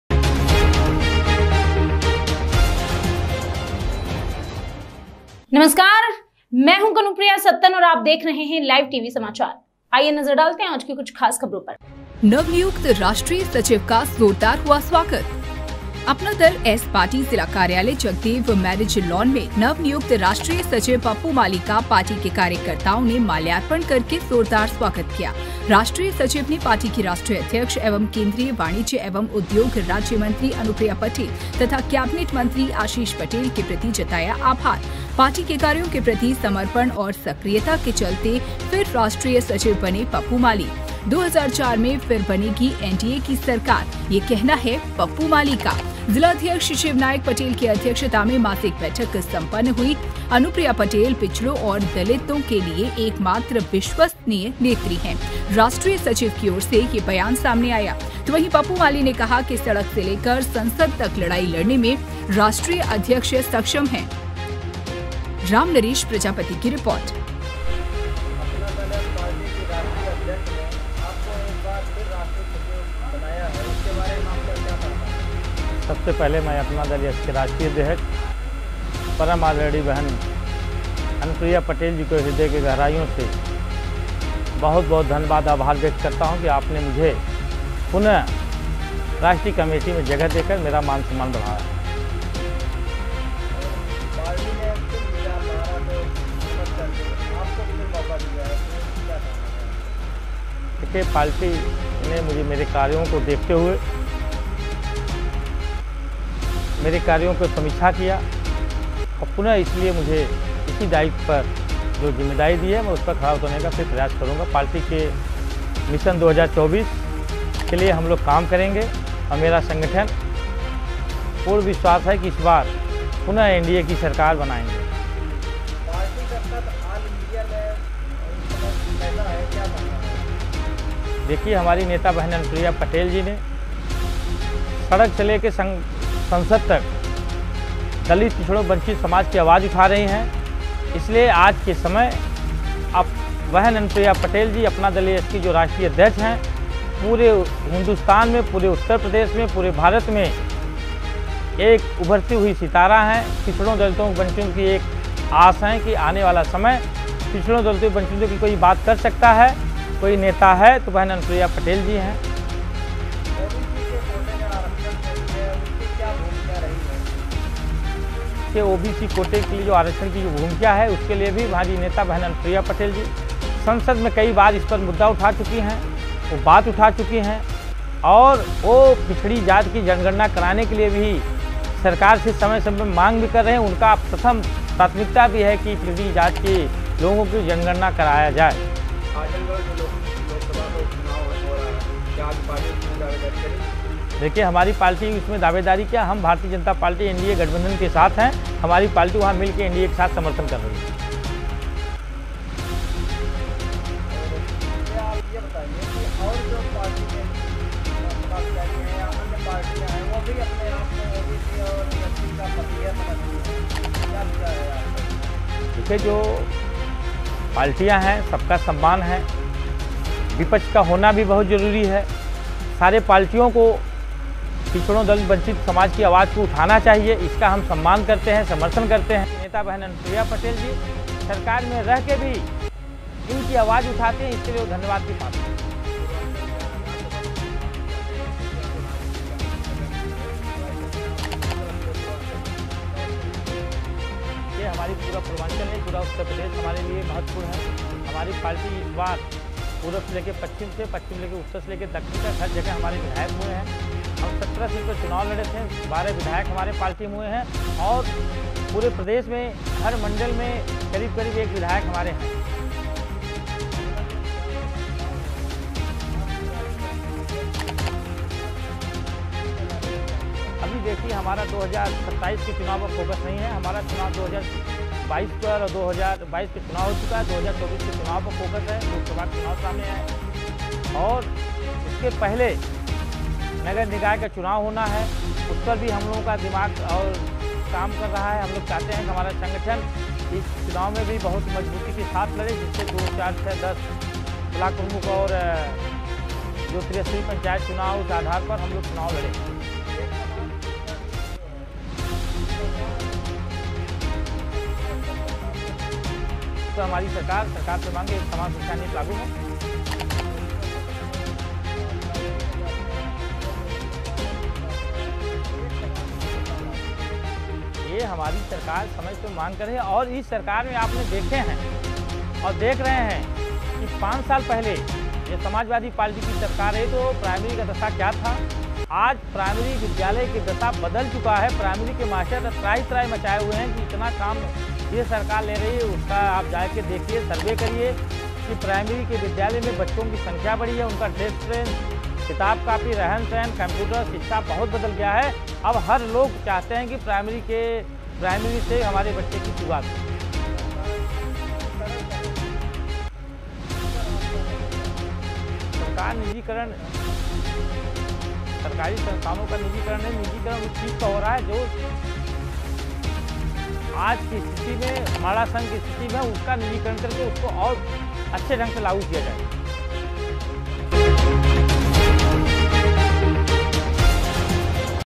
जौनपुर : अपना दल एस पार्टी जिला कार्यालय मैरिज लॉन में नवनियुक्त राष्ट्रीय सचिव का स्वागत किया गया
अपना दल एस पार्टी जिला कार्यालय जगदेव मैरिज लॉन में नवनियुक्त राष्ट्रीय सचिव पप्पू माली का पार्टी के